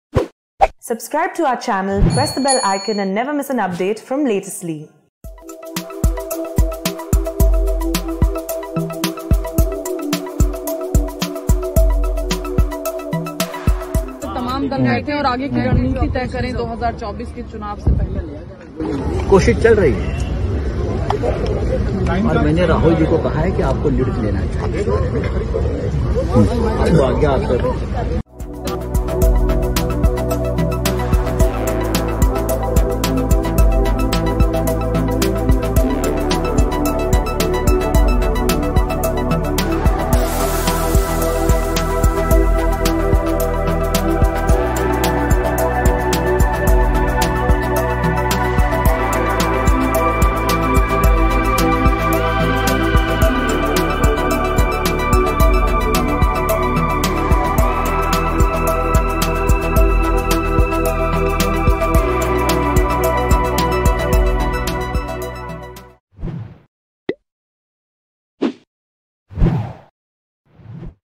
Sanjay Raut Meets Rahul Gandhi: शिवसेना-काँग्रेसमधील जवळीक वाढताना दिसतेय
शिवसेना खासदार संजय राऊत यांनी राहुल गांधी यांची भेट घेतली आहे. प्रियांका गांधींची भेट घेणार आहेत. गोवा आणि उत्तर प्रदेश निवडणुकीत उतरण्यावर शिवसेना आग्रही असताना, या भेटी  महत्वाच्या ठरणार आहे.